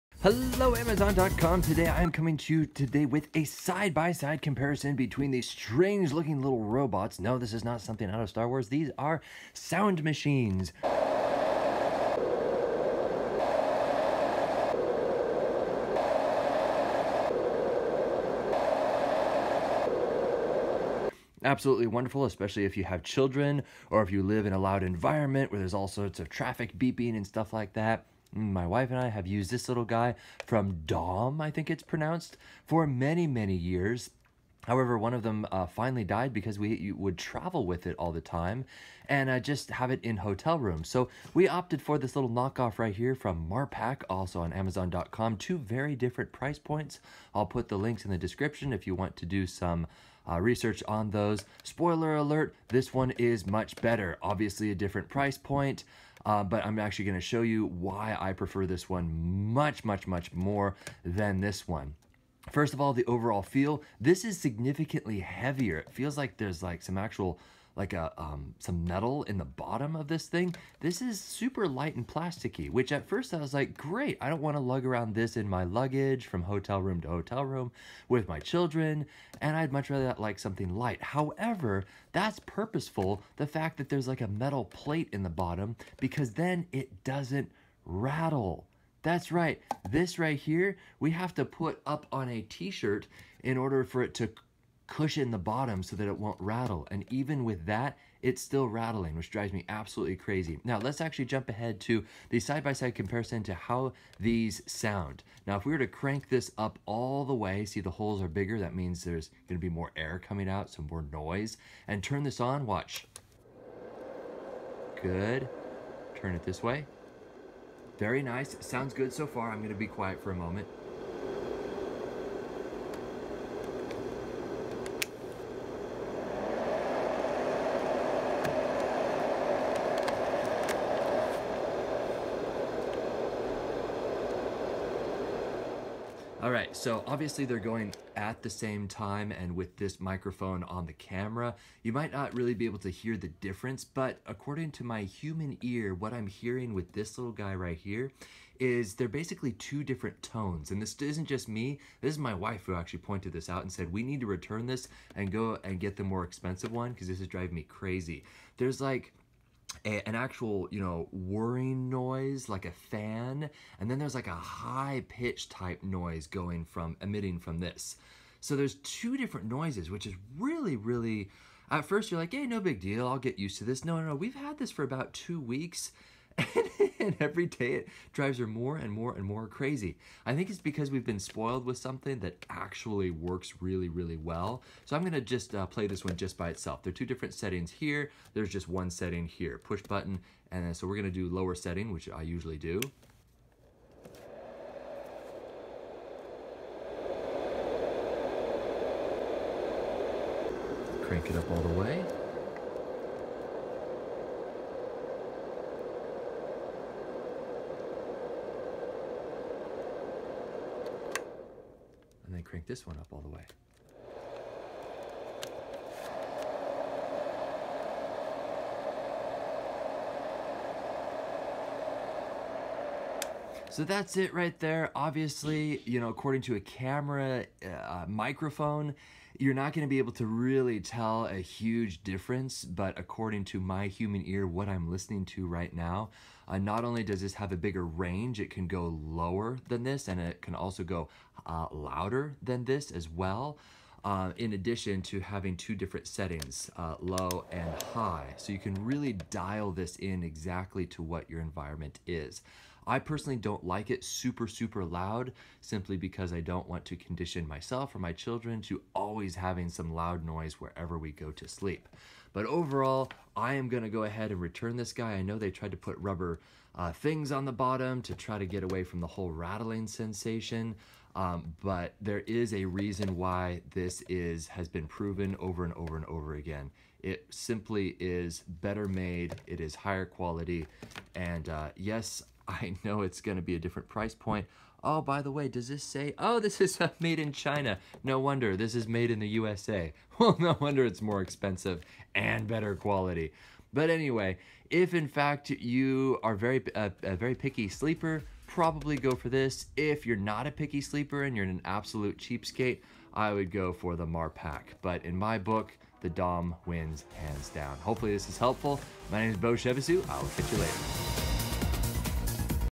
★★★★★ Side-by-side Sound Test: Marpac Dohm Review, Classic white noise sound machine UNO
Not the knock-off.
Update on knock-off sound machine: It's been two weeks, and I thought for sure the plastic, factory odor would go away. It has not.  Turn it on, give it a whiff, and you'll know what I'm talking about.

Video review and test (side-by-side).
We've used both sound machines. There is a big difference.